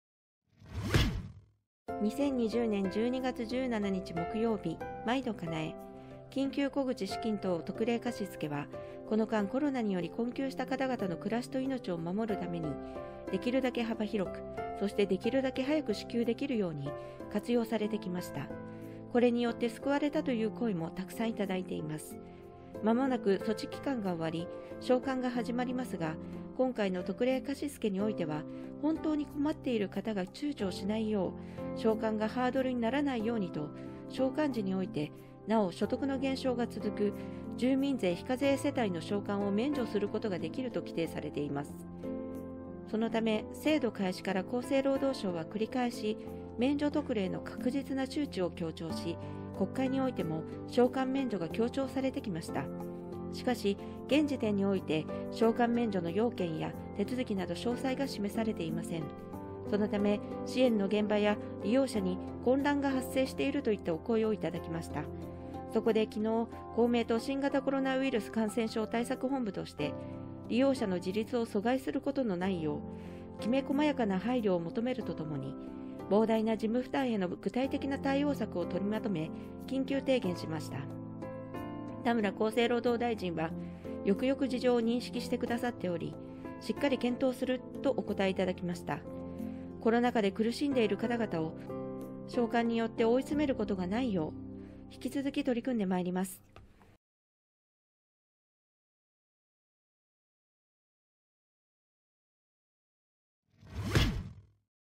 201217まいどかなえ
2020年12月17日木曜日　まいど!かなえ

緊急小口資金等特例貸付は、この間、コロナにより困窮した方々の暮らしと命を守るためにできるだけ幅広く、そしてできるだけ早く支給できるように活用されてきました。これによって救われたという声も沢山いただいてます。

まもなく措置期間が終わり、償還が始まりますが、今回の特例貸付においては本当に困っている方が躊躇しないよう、償還がハードルにならないようにと、「償還時において、なお所得の減少が続く住民税非課税世帯の償還を免除することができる」と規定されています。

そのため、制度開始から、厚生労働省は繰り返し免除特例の確実な周知を強調し、国会においても償還免除が強調されてきました。

しかし、現時点において償還免除の要件や手続きなど詳細が示されていません。そのため、支援の現場や利用者に混乱が発生しているといったお声をいただきました。そこで、昨日、公明党新型コロナウイルス感染症対策本部として、利用者の自立を阻害することないよう、きめ細やかな配慮を求めるとともに、膨大な事務負担への具体的な対応策をとりまとめ、緊急提言しました。

田村厚生労働大臣はよくよく事情を認識して下さっており、「しっかり検討する」とお答えいただきました。

コロナ禍で苦しんでいる方々を償還によって追い詰めるようなことがないよう、引き続き取り組んでまいります。

緊急小口など返済、配慮きめ細かく
免除の要件を提案／厚労相に公明

2020/12/17 １面

　公明党の新型コロナウイルス感染症対策本部の高木美智代事務局長（衆院議員）は１６日、厚生労働省で田村憲久厚労相と山本博司厚労副大臣（公明党）に対し、コロナ禍で収入が減った世帯の生活を支える「緊急小口資金」などの特例貸し付けの返済（償還）に関して、自立を阻害しないよう、きめ細かな配慮を求める緊急提言を申し入れた。田村厚労相は「しっかり検討したい」と応じた。大口善徳衆院議員と山本香苗参院議員が同席した。

　提言では、来年３月末まで特例貸し付けの期限延長が決定された一方、返済免除について要件の詳細が示されていないため、「利用者や支援の現場で混乱が生じている」として、速やかな対応を要請した。

　その上で、具体的な返済免除要件の提案として、住民税非課税世帯の一括免除を求め、返済している期間のうちに非課税世帯となった場合には、それ以降を返済免除とするよう訴えた。非課税世帯ではなく、免除の対象にならなくても、返済が困難な場合には柔軟に対応するよう促した。

　また、窓口となる各市町村の社会福祉協議会で、返済関連などで業務の増大が見込まれることから、対応のために必要な事務費の十分な確保と１０年分の一括交付を提案。併せて返済業務の簡素化なども要望した。